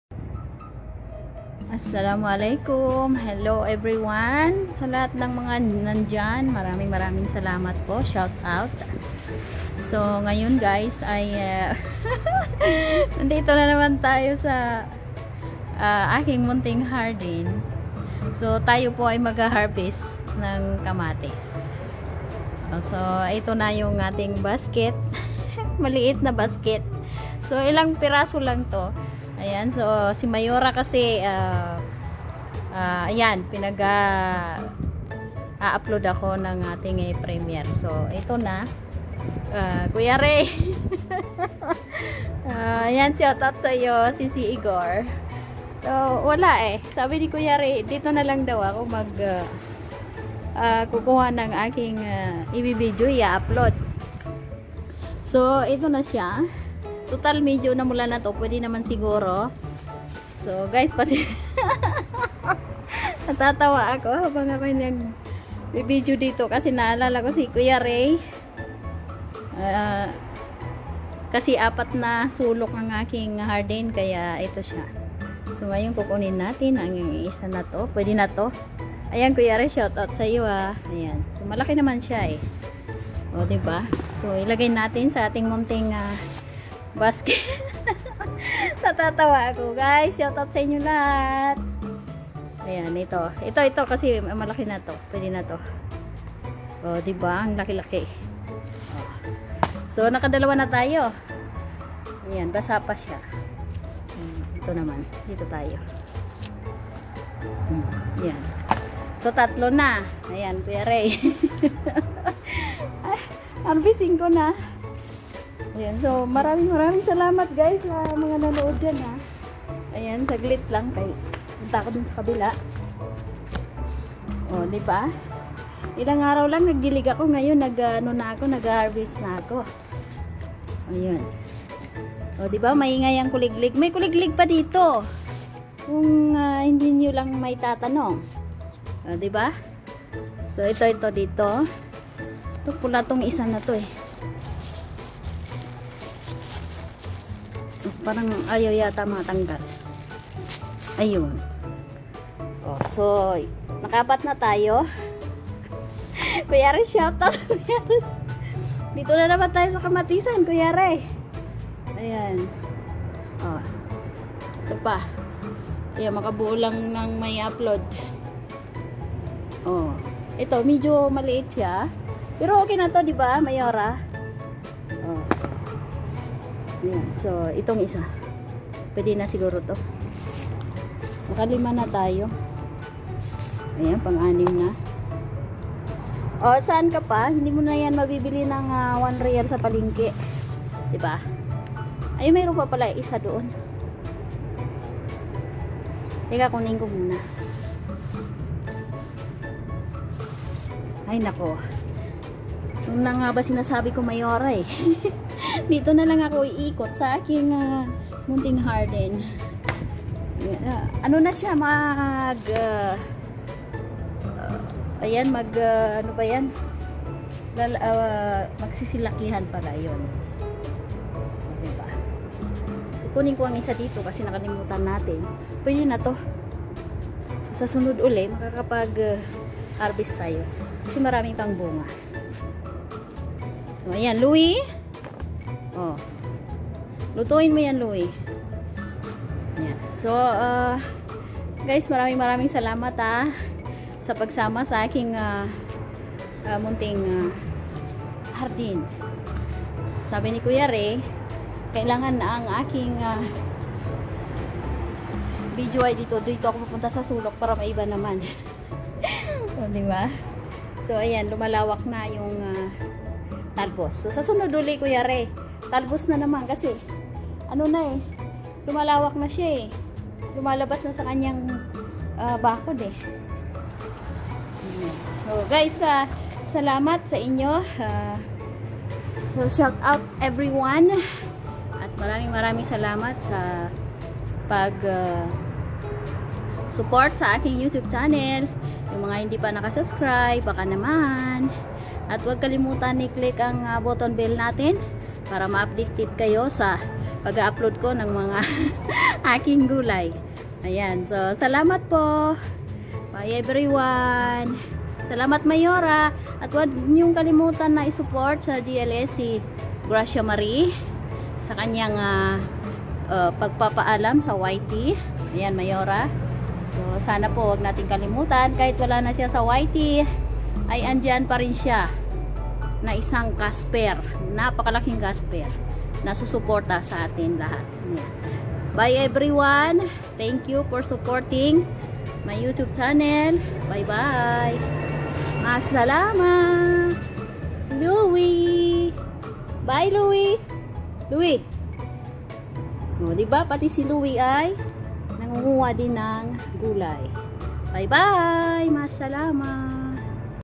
Harvesting Tomatoes From My Small Garden In Saudi Arabia
hello po sa lahat tayo po ngayon ay mag harvest ng kamatis saaking munting harden kaya samahan nyo ako para sa ganon ay makarami tayo ng makuha na kamatis sana po ay nag enjoy kayo sa panonood ng aking video maraming salamat sa lahat sa inyong support ingat po kayo palagi Maassalamah